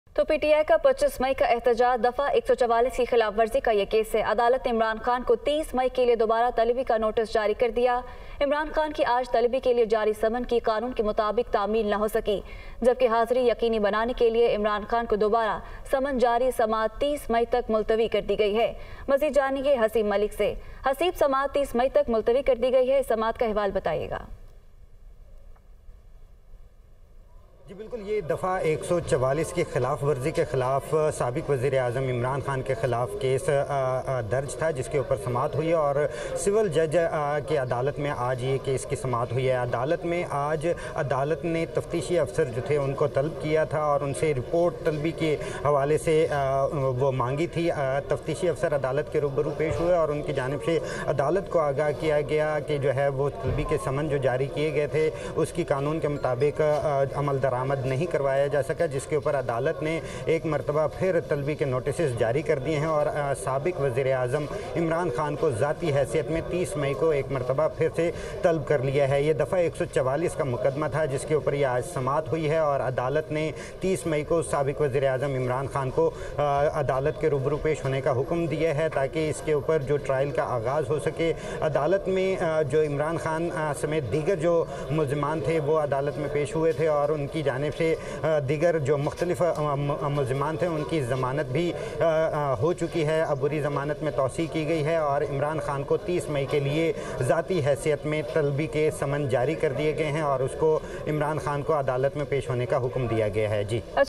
Breaking News!! Imran Khan Phir Adalat Talab | Dunya News
Imran Khan was summoned to court for violation of Section 144 on May 25 protest